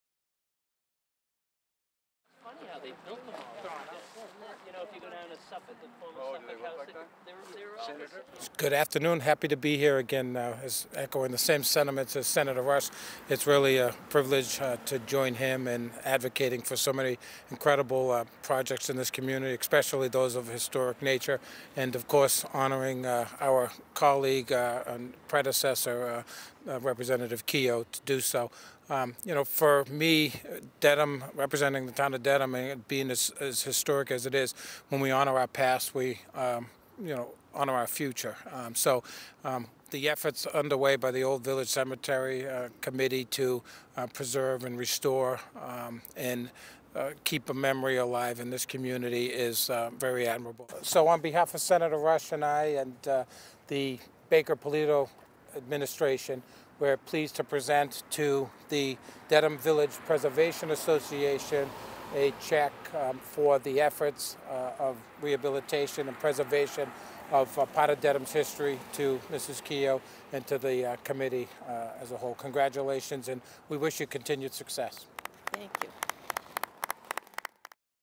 Paul McMurtry: Check Presentation to Old Village Cemetery Committee - Dedham, MA
Dedham, MA - On December 11, 2018,  Representative Paul McMurtry and Senator Mike Rush presented a $250,000 check to Marie-Louise Kehoe, former State Representative and Dedham Selectman, and Chairman of the Old Village Cemetery Restoration Committee. Representative McMurtry commented that “When we honor our past, we honor our future.”